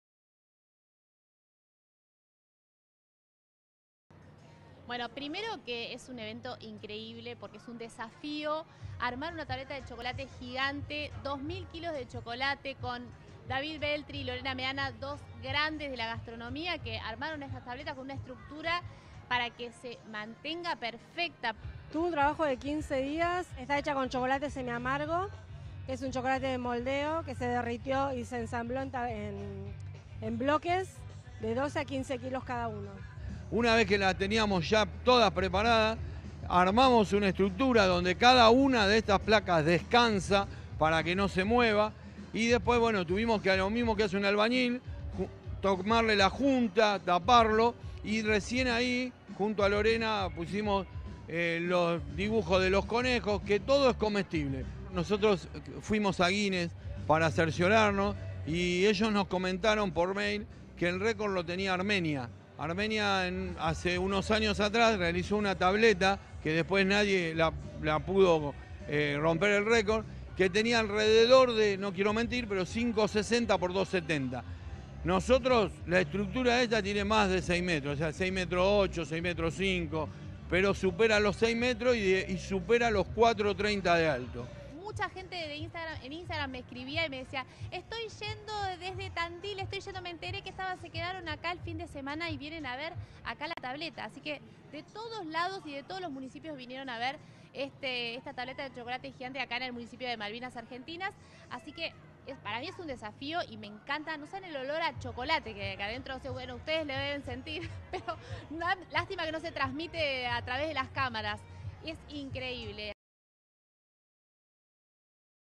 Malvinas festejó la Pascua con el récord mundial de la tableta de chocolate
Malvinas festejó la Pascua con el récord mundial de la tableta de chocolate y un gran show de Los Nocheros

Durante estos cuatro días de Semana Santa se realizó en el distrito el armado de la tableta artesanal de chocolate más grande del mundo, la cual fue visitada por miles de personas. Hasta el momento solo se había realizado en Armenia pero ahora, certificado por escribana, el logro lo tiene Malvinas Argentinas.

El municipio de Malvinas Argentinas ya batió un récord mundial. En los cuatro días del fin de semana largo de Pascuas se realizó la tableta artesanal de chocolate más grande del mundo, en una carpa especialmente climatizada que se armó en las inmediaciones del Palacio Municipal. El festejo tuvo como cierre un broche de oro con el recital de Los Nocheros.

La hazaña de la fabricación del chocolate fue dirigida por los creadores de la misma, David Veltri y Lorena Meana. La pieza de chocolate de más de 2.000 kilos, se rompió el domingo por la tarde para ser compartida por todos los presentes. La encargada de hacerlo fue la intendenta municipal Noe Correa, quien estuvo acompañada por el vicepresidente primero de la Cámara de Senadores de la provincia de Buenos Aires, Luis Vivona y la secretaria de Gobierno y Monitoreo Institucional de Malvinas Argentinas, María Luján Salgado.

“David y Lorena trabajaron con mucho amor, como todo lo que hacemos en Malvinas. Estamos muy felices, disfrutando en familia, en un día que será inolvidable para todos nosotros”, aseguró la jefa comunal.

Por su parte, Lorena Meana, dijo: “Somos récord mundial. Mucha emoción, estamos muy contentos y agradecidos al municipio que nos convocó, por la invitación y el hacernos parte. La organización fue un lujo y la pasamos bárbaro”.

“Estoy muy contento, con muchos sentimientos encontrados. El chocolate es muy delicado asique fue todo un desafío, días sin dormir. Pero lo logramos, Malvinas tiene un récord y podemos sentirnos orgullosos de eso”, sumó David Veltri.

La tableta fue certificada por escribana. Durante estos cuatro días por los que pasaron miles de personas, hubo además actividades infantiles como decoración de galletas, chupaletas y taller de arte.

También estuvieron presentes profesionales de la cocina como el pastelero Mauricio Asta y Jimena Monteverde. Por último, el gran cierre estuvo a cargo de Los Nocheros quienes brindaron un gran show musical.